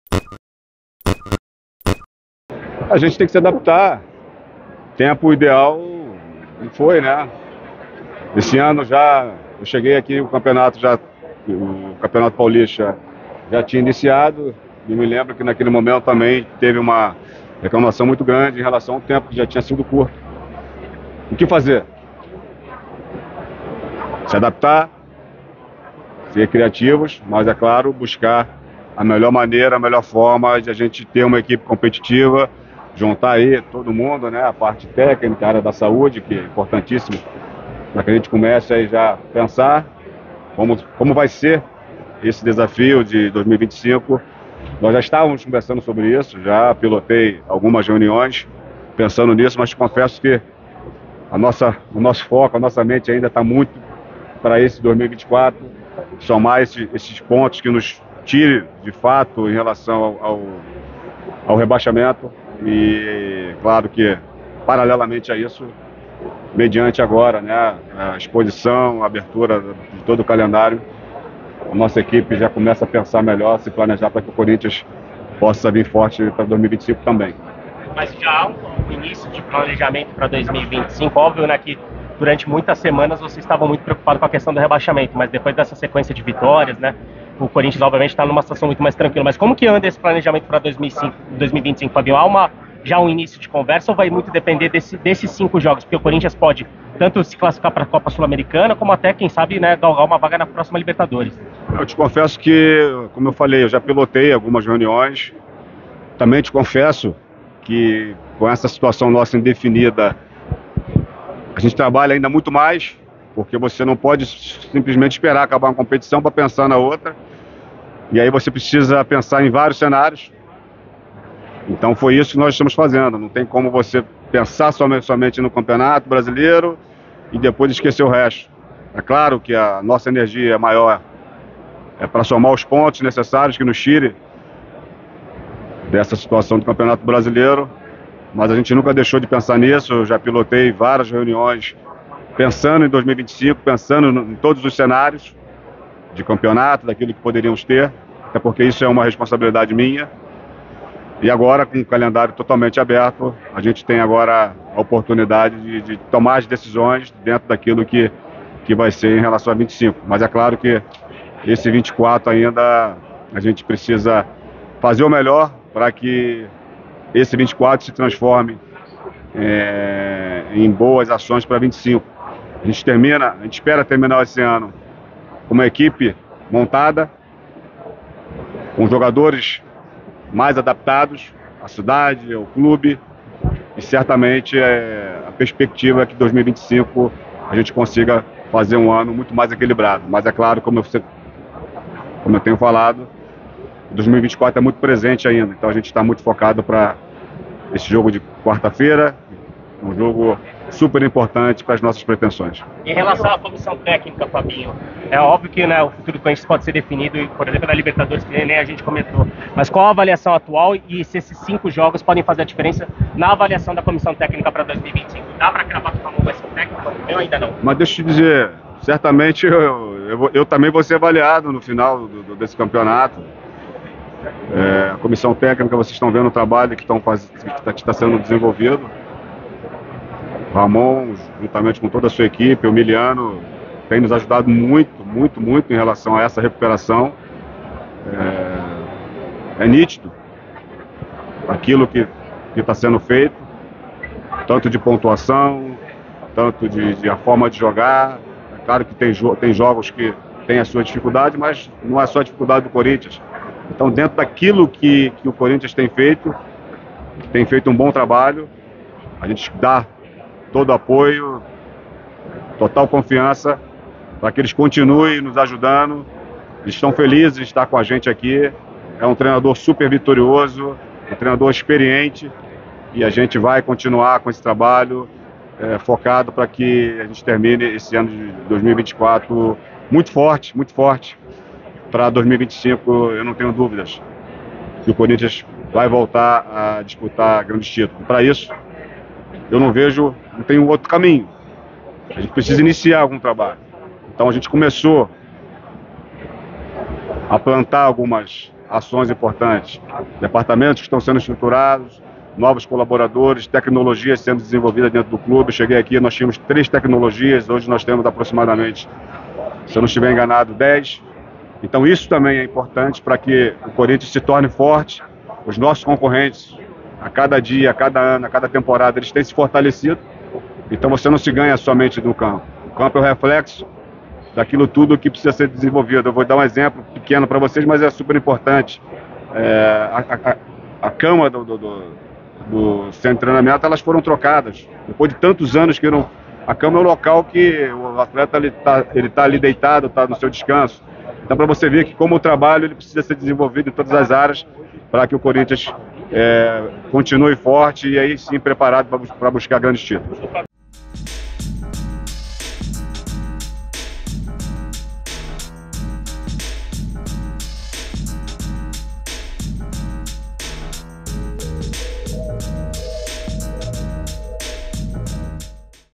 FABINHO SOLDADO ABRE O JOGO SOBRE RAMÓN DÍAZ E PLANEJAMENTO DO CORINTHIANS PARA 2025
Em entrevista após sorteio do Paulistão 2025, Fabinho Soldado, executivo de futebol do Corinthians, falou sobre o planejamento do Timão para a próxima temporada. Confira!